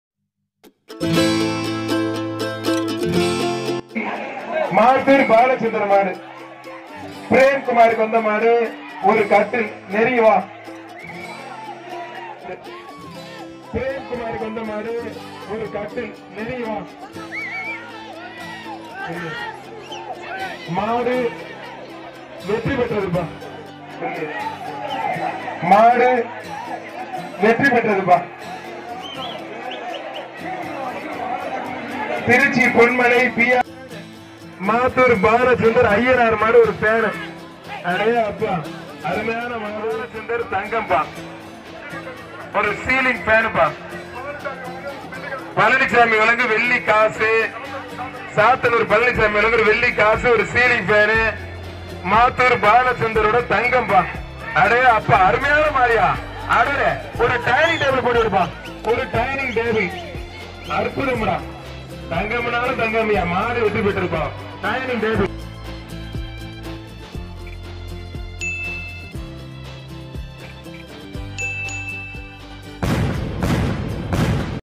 மாத்தூர் பாலச்சந்தர் அவர்களின் காளைகள் அடுத்தடுத்து எல்லாமே வெற்றி தான்
புதுக்கோட்டை மாவட்டம் மாத்தூர் பாலச்சந்தர் அவர்களின் காளைகள் { 3 } அடுத்தடுத்து ..
ஜல்லிக்கட்டு போட்டிக்கு எங்கு சென்றாலும் அந்த இடத்தில் வெற்றி வாகை சூடி கொண்டுவருகிறது அதன் தொகுப்பை உங்களுக்கு முழுவதுமாக காணவே இந்த காணொளி...